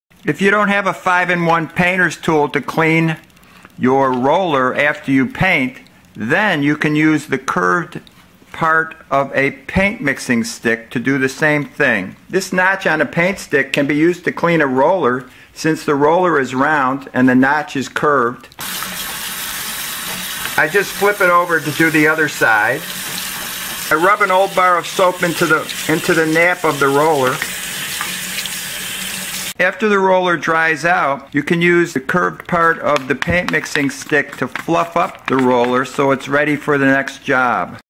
Use a Paint Mixing Stick to Clean a Paint Roller
No need for a 5 in 1 tool to clean a roller. You already have a paint stick.